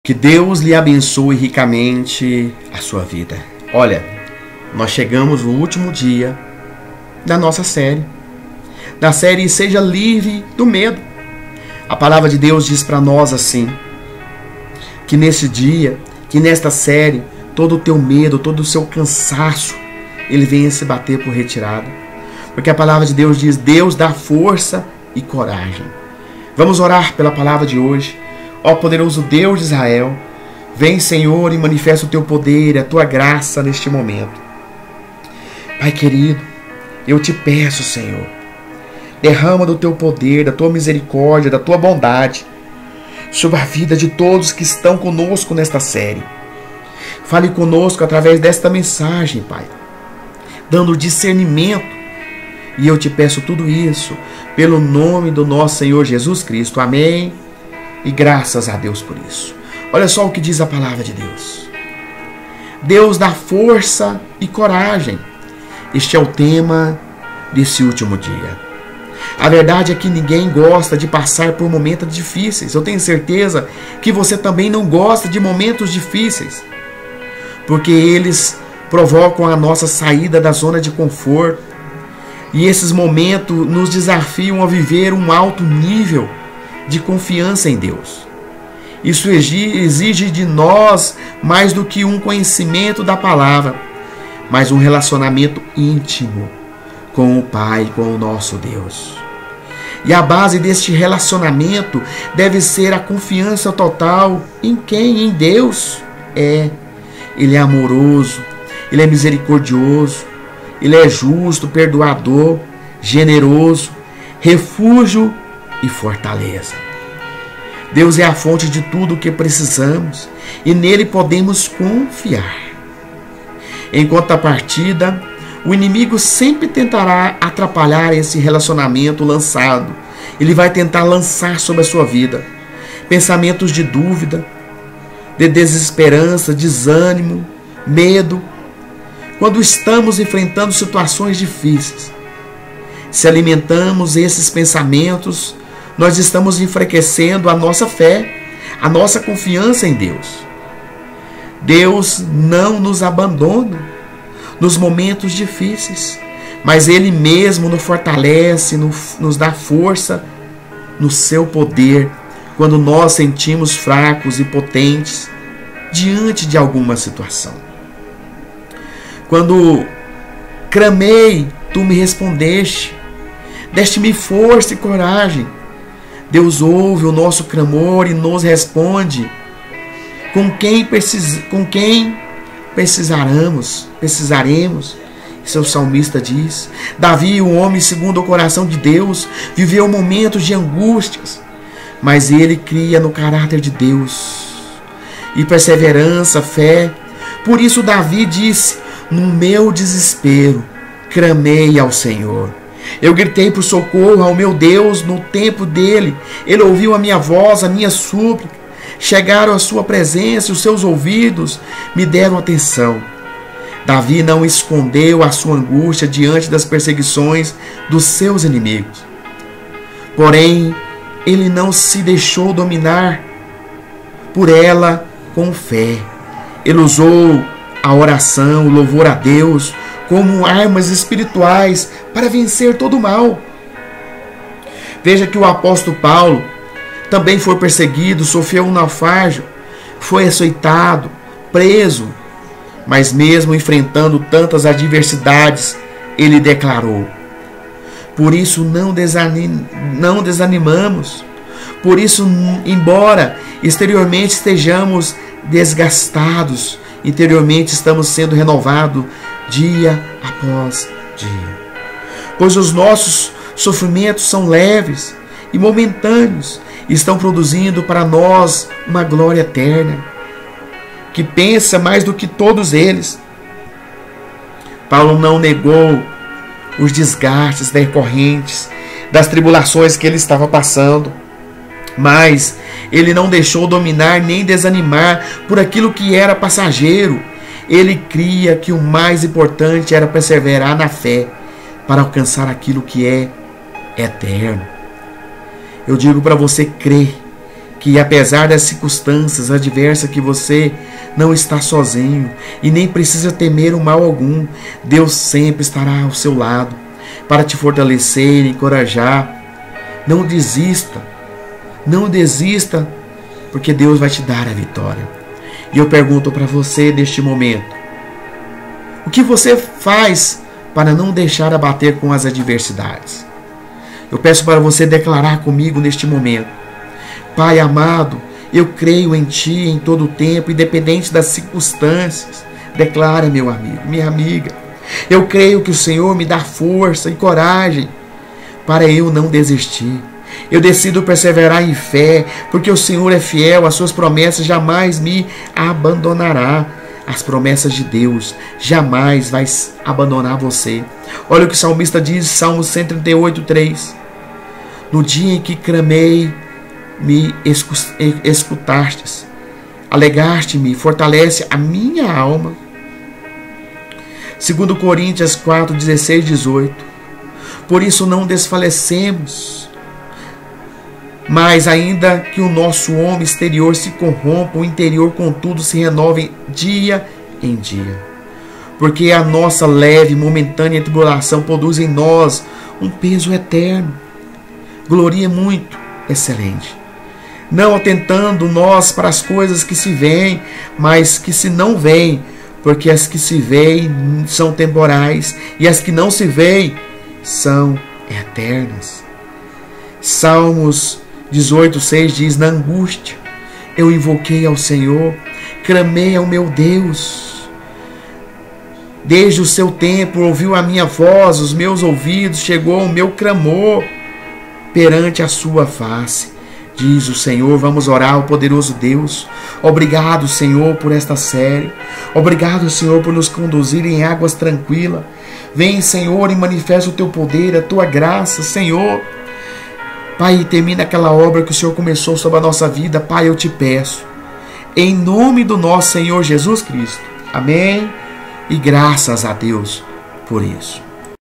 Série | Seja Livre do Medo | Deus te dá Força e Coragem | 5/5 | Rodrigo Morais
DEUS TE DÁ FORÇA E CORAGEM – Dia 5
A verdade é que ninguém gosta de passar por momentos difíceis, porque provocam a nossa saída da zona de conforto e nos desafiam a viver um alto nível de confiança em Deus. Isso exige de nós mais do que um conhecimento da Palavra, mas um relacionamento íntimo com o Pai. E a base desse relacionamento deve ser a confiança total em quem Deus é: amoroso, misericordioso, justo, perdoador, generoso, refúgio e fortaleza. Deus é a fonte de tudo o que precisamos e nele podemos confiar.

Em contrapartida, o inimigo sempre tentará atrapalhar esse relacionamento, lançando pensamentos de dúvida, desesperança, desânimo e medo quando estivermos enfrentando situações difíceis. Se alimentarmos esses pensamentos, enfraqueceremos a nossa fé e a nossa confiança em Deus.

Deus não nos abandona nos momentos difíceis, mas Ele mesmo nos fortalece na força do seu poder quando nos sentimos fracos e impotentes diante de alguma situação. "Quando clamei, tu me responde; deste-me força e coragem." Deus ouve o nosso clamor e nos responda com o que precisamos.

Davi, o homem segundo o coração de Deus, viveu momentos de angústia, mas ele criou no caráter de Deus e perseverava em fé. Por isso, Davi disse: "No meu desespero clamei ao SENHOR; gritei por socorro ao meu Deus. Do seu templo Ele ouviu a minha voz; minhas súplicas chegaram à sua presença e seus ouvidos me deram atenção". Davi não esconde sua angústia diante das perseguições de seus inimigos. Porém, ele não se deixou dominar por ela e com fé usou a oração e o louvor a Deus como armas espirituais para vencer o mal.

O apóstolo Paulo foi perseguido, sofreu naufrágio, foi açoitado e preso, mas mesmo enfrentando tantas adversidades, ele declarou: "Por isso não desanimamos. Embora exteriormente nos preocupemos em nos desgastar, interiormente estamos sendo renovados dia após dia, pois os nossos sofrimentos leves e os momentos estão produzindo para nós uma glória eterna que pesa mais do que todos eles.” Paulo não negou os desgastes decorrentes das tribulações, mas ele não se deixou dominar e nem desanimar por aquilo que era passageiro.Ele criou que o mais importante foi perseverar em fé para alcançar aquilo que é eterno.

Creia que, apesar das circunstâncias adversárias, você não está sozinho e nem precisa temer mal algum. Deus sempre estará ao seu lado para te fortalecer, encorajar a não desistir e te dar a vitória.

O QUE VOCÊ FAZ PARA NÃO SE DEIXAR ABATER COM AS ADVERSIDADES?
DECLARAÇÃO DE FÉ: Pai Amado, eu confio em Ti em todo tempo. Independente das situações, eu creio que o Senhor me dá força e coragem para eu não desistir. Eu decidi perseverar em fé, porque o Senhor é fiel às suas promessas e jamais me abandonará

3 Não dia em que eu clamei, me escutaste; alentaste-me, fortalecendo a minha alma.
SALMOS 138:3

2CORÍNTIOS 4:16-18
16 Por isso, não desfalecemos; mas, ainda que o nosso homem exterior se corrompa, o interior, contudo, se renova de dia em dia.
17 Porque a nossa leve e momentânea tribulação produz para nós um peso eterno de glória mui excelente,
18 não atentando nós nas coisas que se veem, mas nas que se não veem; porque as que se veem são temporais, e as que se não veem são eternas.

Na angústia, invoquei ao Senhor e clamei ao meu Deus; desde o seu templo ouviu a minha voz e aos seus ouvidos chegou o meu clamor perante sua face.
SALMOS 18:6

Fonte de estudos:  Life.Church / YouVersion